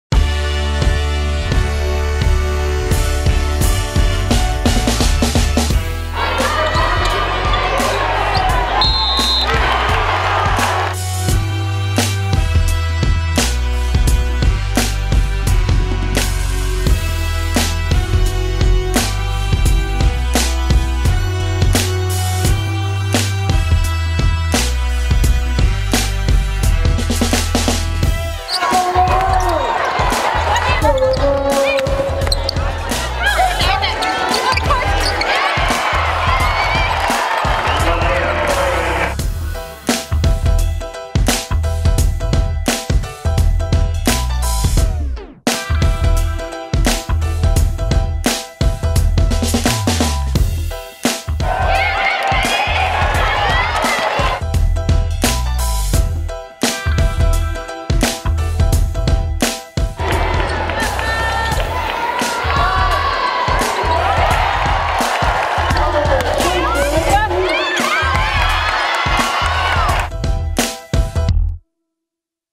Women's Basketball Beats Southern Utah
The day after Thanksgiving, the Lobos dominated Southern Utah, and will play Duquesne in the championship game of the Thanksgiving Tournament.